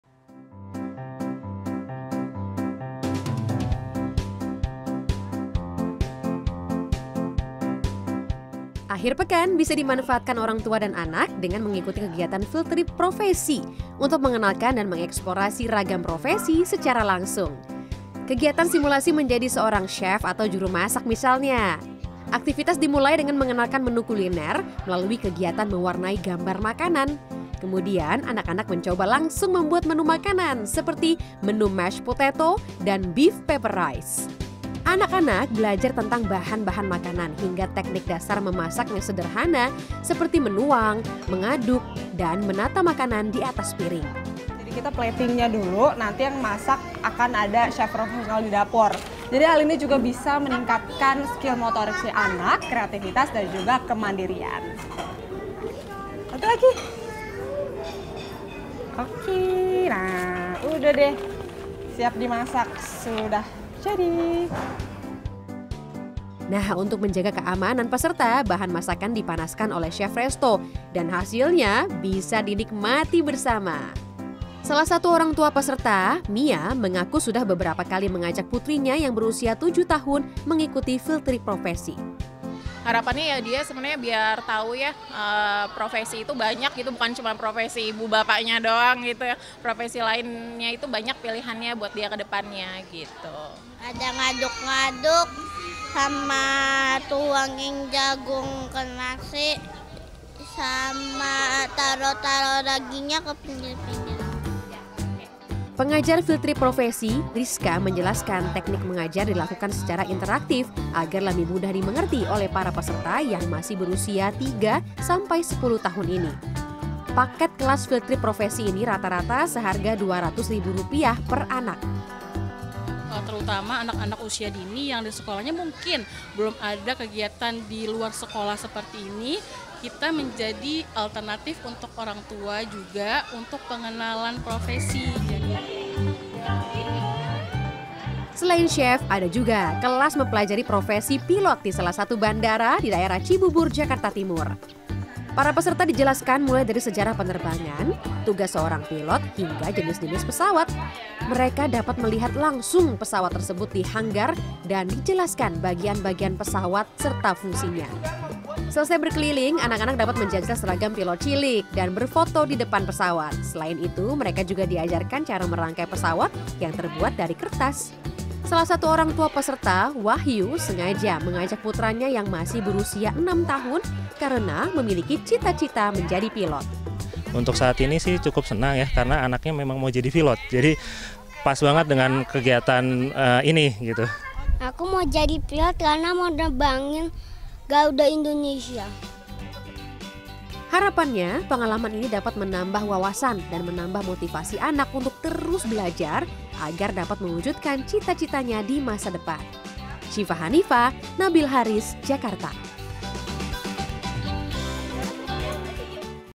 Mengenalkan Ragam Profesi Pada Anak
Kalau sudah besar nanti mau jadi apa? Sepertinya pertanyaan itu yang selalu diberikan untuk anak-anak ya. Impiannya pun pasti macam-macam. Nah supaya anak lebih termotivasi, ajak mereka mengikuti field trip mencoba ragam profesi secara langsung yuk. Seperti dalam liputan ini